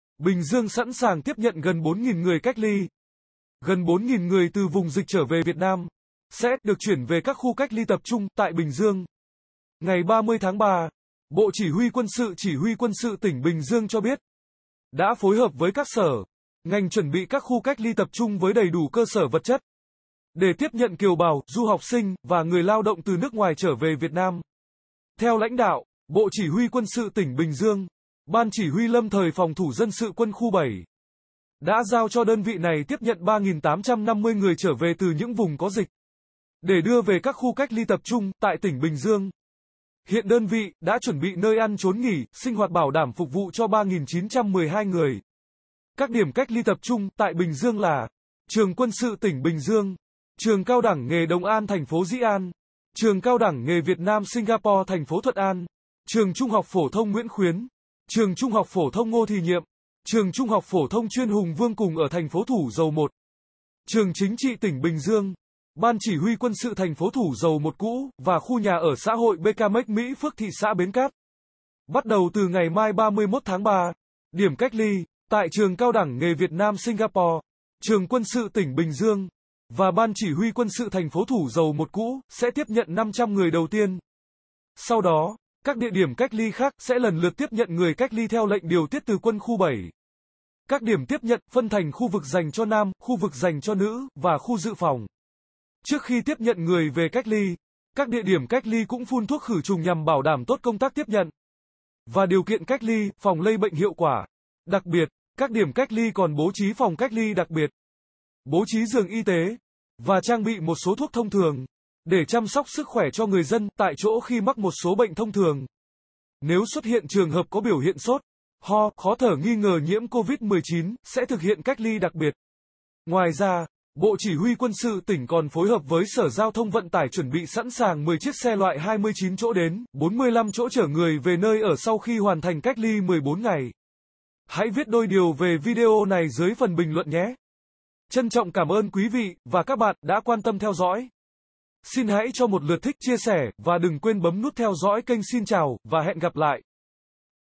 Bình Dương sẵn sàng tiếp nhận gần 4.000 người cách lya
Tải Game Android hay Miễn Phí Đăng kí kênh xem tin tức mới nhất Gần 4.000 người từ vùng dịch trở về Việt Nam sẽ được chuyển về các khu cách ly tập trung tại Bình Dương.Ngày 30-3, Bộ Chỉ huy quân sự (CHQS) tỉnh Bình Dương cho biết đã phối hợp với các sở, ngành chuẩn bị các khu cách ly tập trung với đầy đủ cơ sở vật chất để tiếp nhận kiều bào, du học sinh và người lao động từ nước ngoài trở về Việt Nam.Theo lãnh đạo, Bộ CHQS tỉnh Bình Dương, Ban Chỉ huy lâm thời phòng thủ dân sự Quân khu 7 đã giao cho đơn vị này tiếp nhận 3.850 người trở về từ những vùng có dịch để đưa về các khu cách ly tập trung tại tỉnh Bình Dương.Hiện đơn vị đã chuẩn bị nơi ăn chốn nghỉ, sinh hoạt bảo đảm phục vụ cho 3.912 người.Các điểm cách ly tập trung tại Bình Dương là: Trường Quân sự tỉnh Bình Dương, Trường CĐ nghề Đồng An (TP Dĩ An), Trường CĐ nghề Việt Nam - Singapore (TP Thuận An), Trường THPT Nguyễn Khuyến, Trường THPT Ngô Thì Nhiệm, Trường THPT chuyên Hùng Vương (cùng ở TP Thủ Dầu Một), Trường Chính trị tỉnh Bình Dương, Ban CHQS TP Thủ Dầu Một (cũ) và khu nhà ở xã hội Becamex - Mỹ Phước (thị xã Bến Cát).Bắt đầu từ ngày mai (31-3), điểm cách ly tại Trường CĐ nghề Việt Nam - Singapore, Trường Quân sự tỉnh Bình Dương và Ban CHQS TP Thủ Dầu Một (cũ) sẽ tiếp nhận 500 người đầu tiên.Sau đó, các địa điểm cách ly khác sẽ lần lượt tiếp nhận người cách ly theo lệnh điều tiết từ Quân khu 7.Các điểm tiếp nhận, phân thành khu vực dành cho nam, khu vực dành cho nữ và khu dự phòng.Trước khi tiếp nhận người về cách ly, các địa điểm cách ly cũng phun thuốc khử trùng nhằm bảo đảm tốt công tác tiếp nhận và điều kiện cách ly, phòng lây bệnh hiệu quả.Đặc biệt, các điểm cách ly còn bố trí phòng cách ly đặc biệt, bố trí giường y tế và trang bị một số thuốc thông thường để chăm sóc sức khỏe cho người dân tại chỗ khi mắc một số bệnh thông thường.Nếu xuất hiện trường hợp có biểu hiện sốt, ho, khó thở nghi ngờ nhiễm COVID-19, sẽ thực hiện cách ly đặc biệt.Ngoài ra, Bộ CHQS tỉnh còn phối hợp với Sở Giao thông Vận tải chuẩn bị sẵn sàng 10 chiếc xe (loại 29 chỗ đến 45 chỗ) chở người về nơi ở sau khi hoàn thành cách ly 14 ngày.